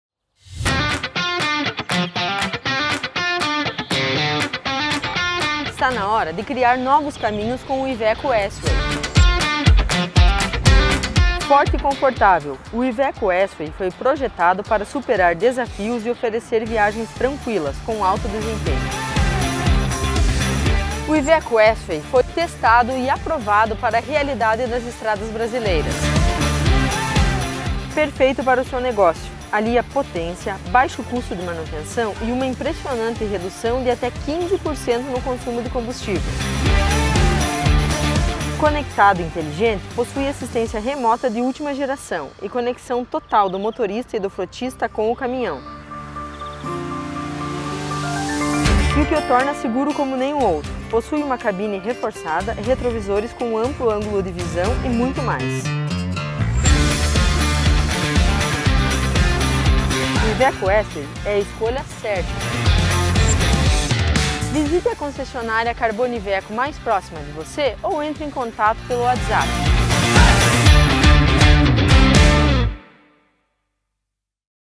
S-Way é a escolha inteligente para quem quer ir mais longe!
O IVECO S-Way é um caminhão robusto, confortável e tecnológico, que entrega a versatilidade máxima para atender todas as demandas de operações da sua empresa! 💙⚡